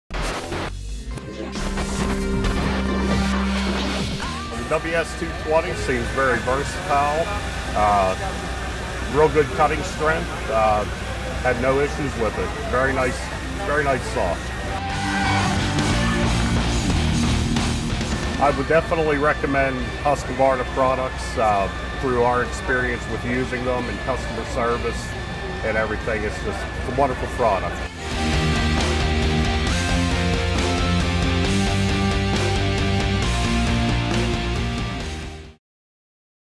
WOC 2014 Customer interview #16 - Chris Winghart
Hear why Chris Winghart from the Concrete Coring Company, would recommend Husqvarna Construction Products and what his comments are from trying the new WS 220 wall saw.